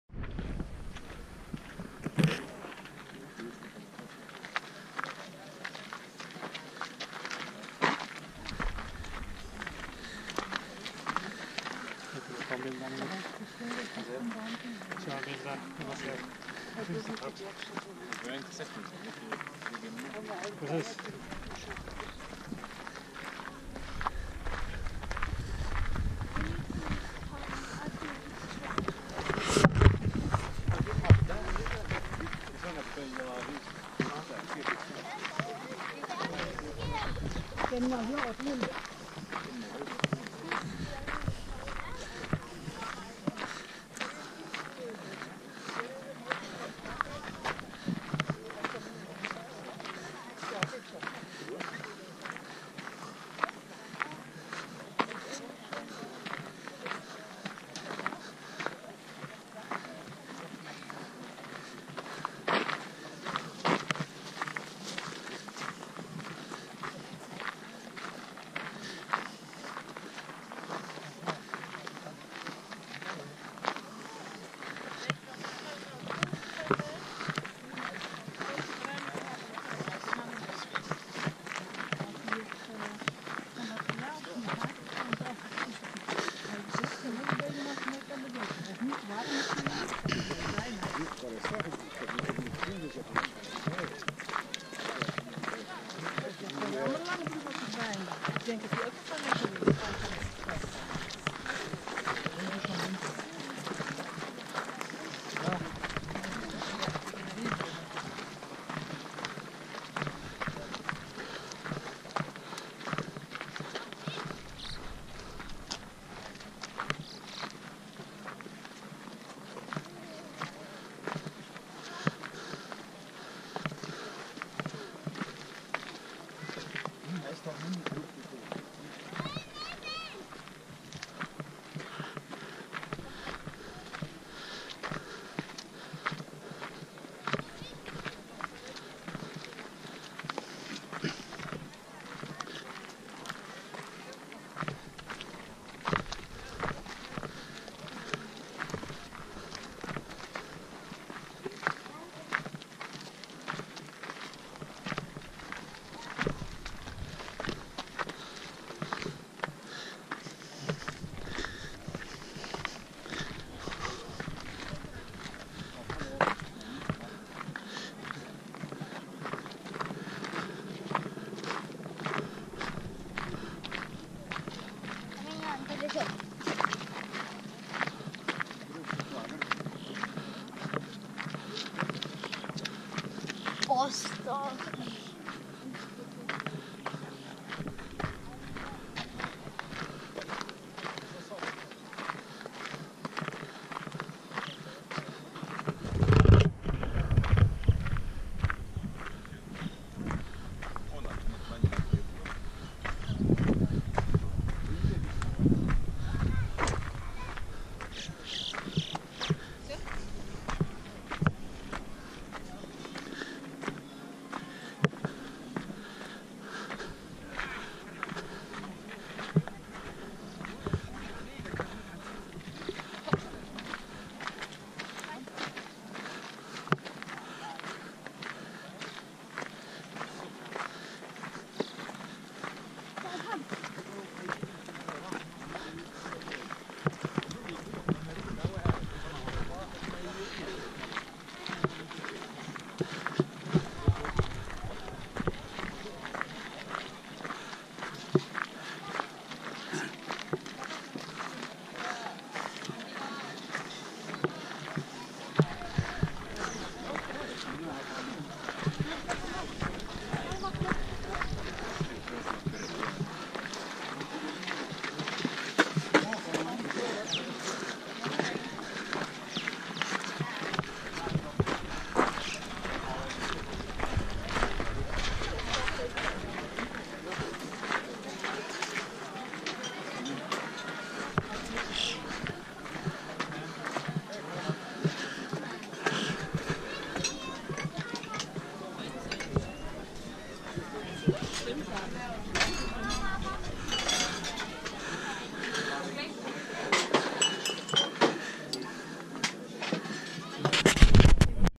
Wandeling Adelaarsnest Berchtesgaden Duitsland
Je kan met de originele gouden lift naar boven en wandelend naar beneden lopen. Natuur, uitzicht en gescheidenis beleven.